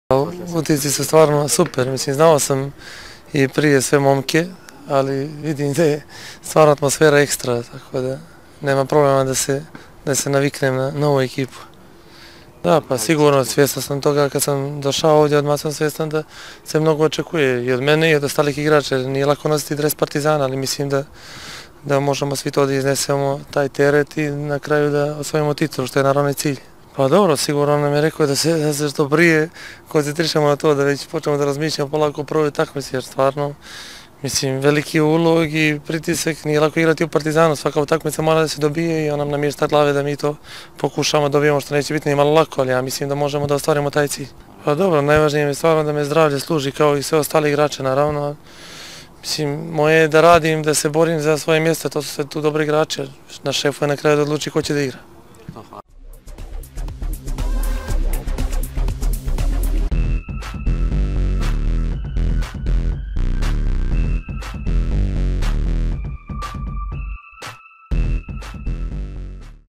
Škuletić (Partizan): Svestan sam pritiska
Novajlija i ofanzivac u redovima Partizana Petar Škuletić konstatovao je ad se dobro privikao na crno-beler trenerke i naglasio da je svestan pritiska koji sa sobom nosi grb šestostrukog uzastopnog prvaka države. Obećao je veliku borbu i golove koji se od njega i očekuju...